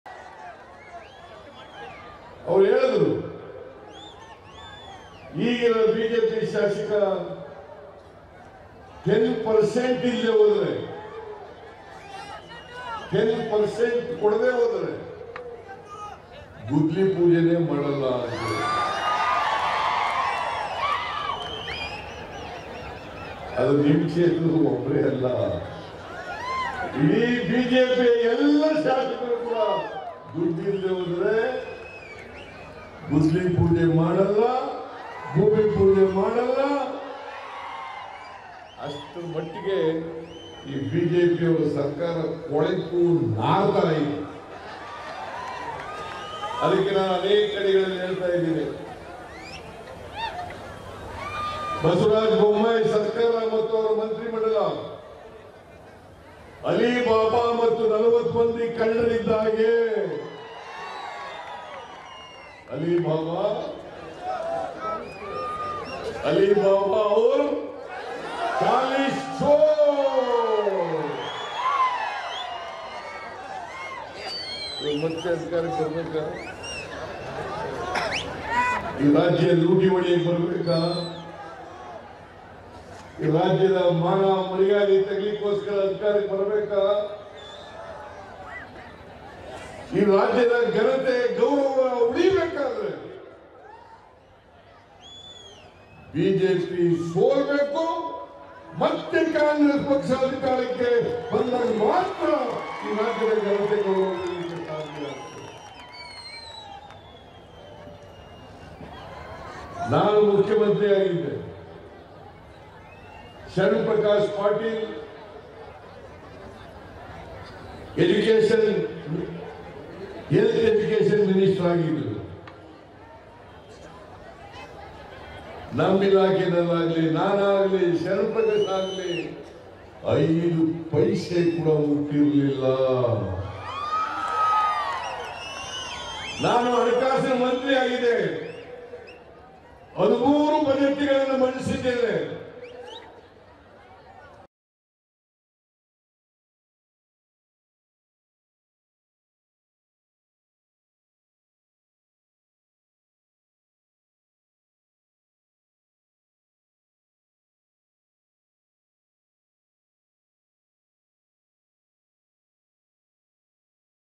10% ಇಲ್ಲಾ ಅಂದ್ರೆ ಬಿಜೆಪಿ ಶಾಸಕರು ಗುದ್ದಲಿ ಪೂಜೆನೂ ಮಾಡಲ್ಲ..! | Siddaramaiah | BJP | Tv5 Kannada
10% ಇಲ್ಲಾ ಅಂದ್ರೆ ಬಿಜೆಪಿ  ಶಾಸಕರು ಗುದ್ದಲಿ ಪೂಜೆನೂ ಮಾಡಲ್ಲ..! | Siddaramaiah | BJP | Tv5 Kannada